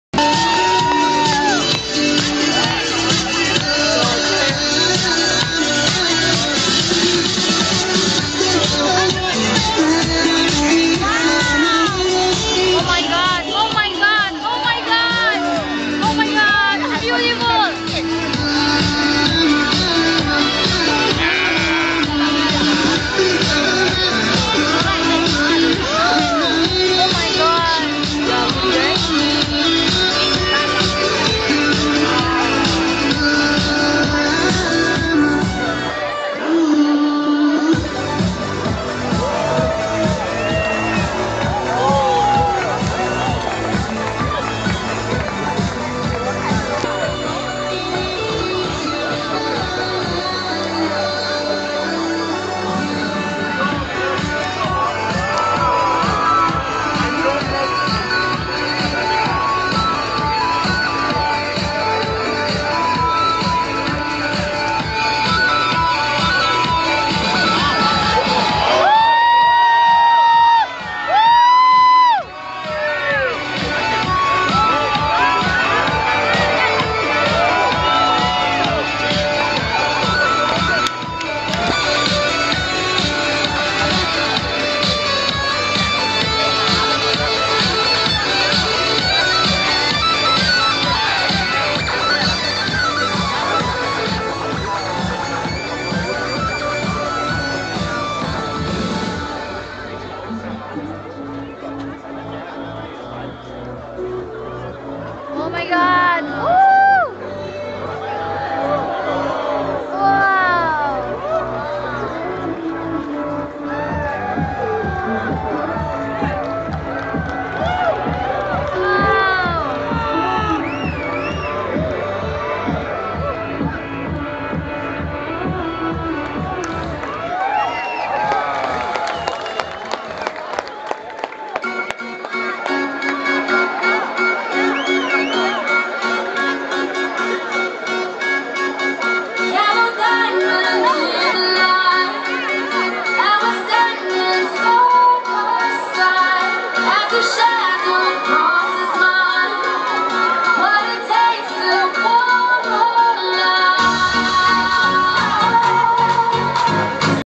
Fire Dance at Kempinski Hotel Nay Pyi Taw Myanmar
Amazing night i've been see in Myanmar what a great performance from Kempinski Hotel Nay Pyi Taw Myanmar